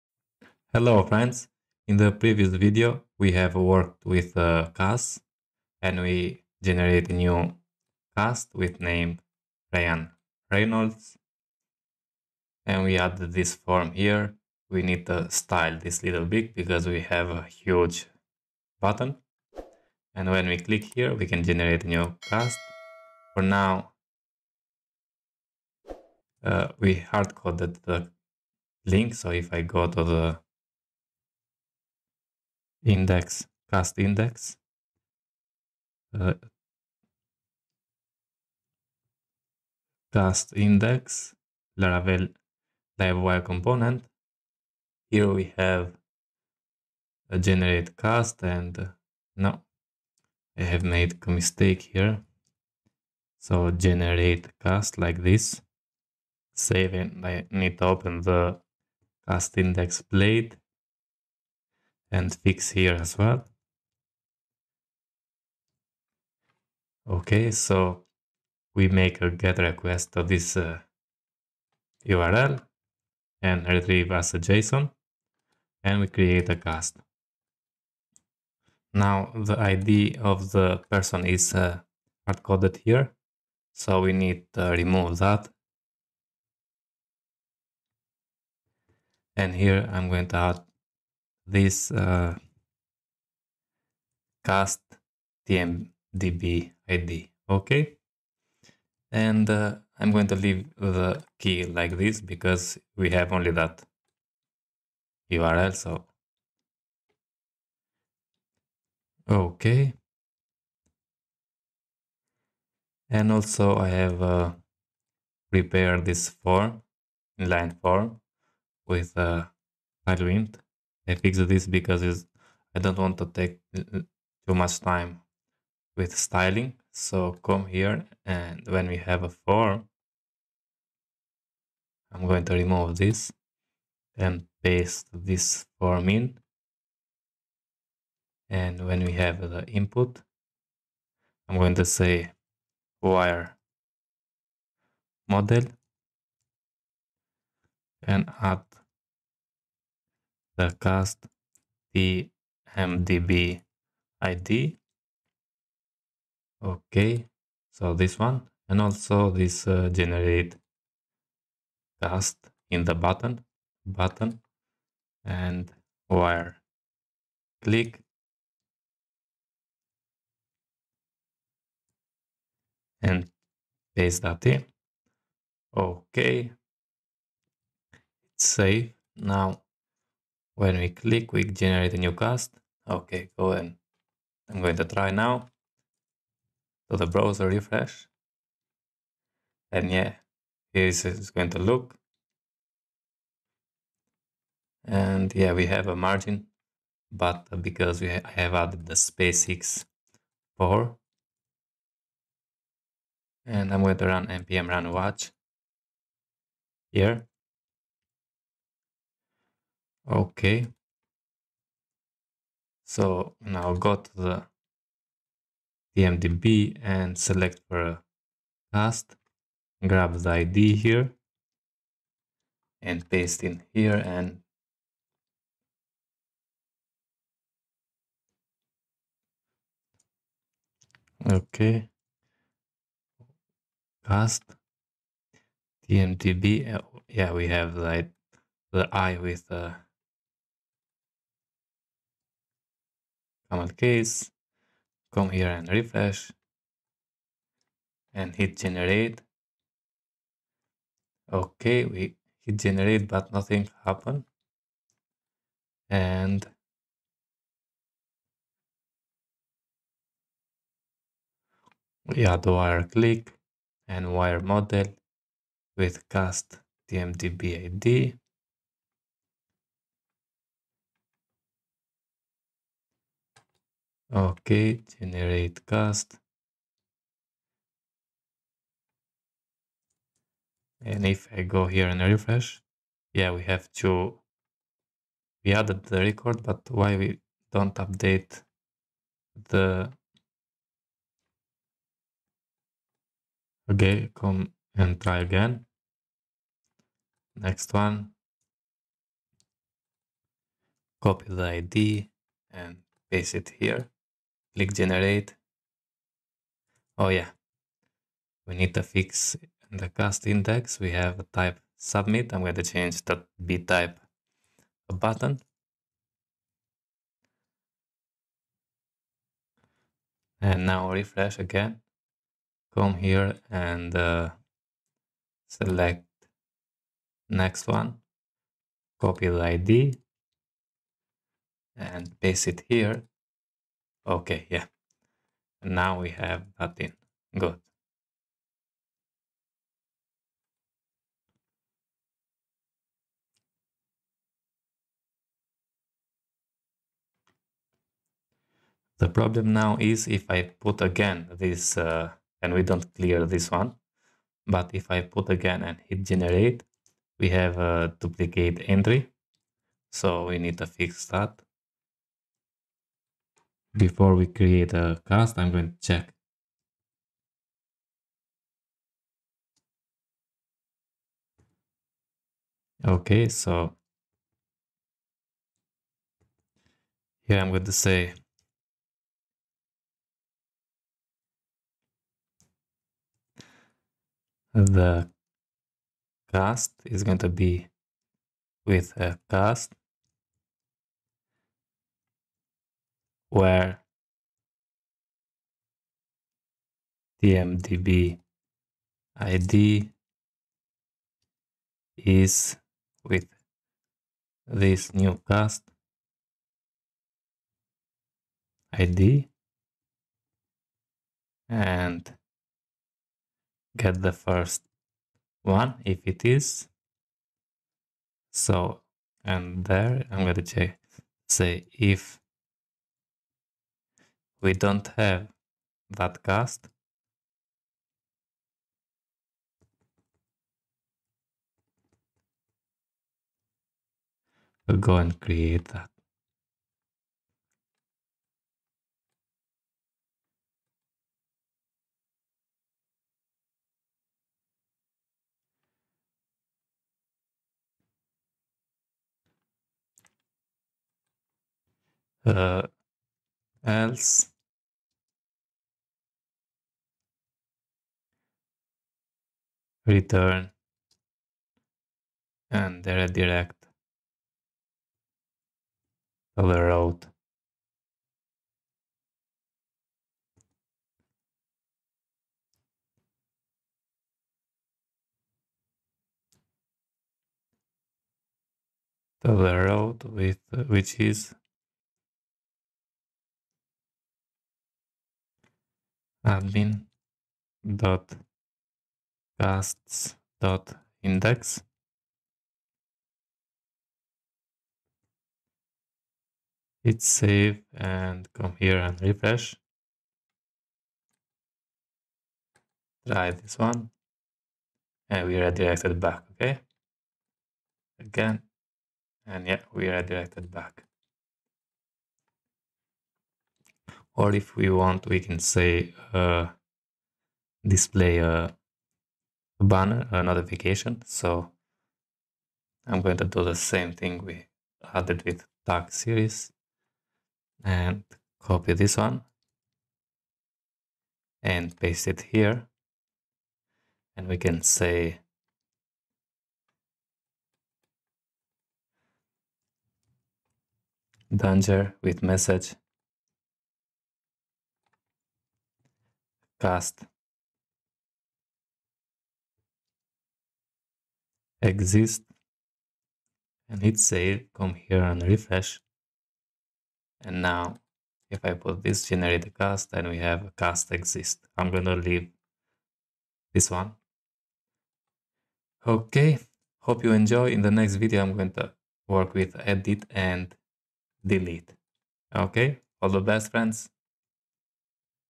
Laravel Livewire Tutorial Create Movie Website #13 Fix Generate Cast From Api
In this video, we are going to work with casts.
Laravel movie website
laravel livewire movie website
Add Tailwind Css Admin Dashboard
add tailwind css responsive table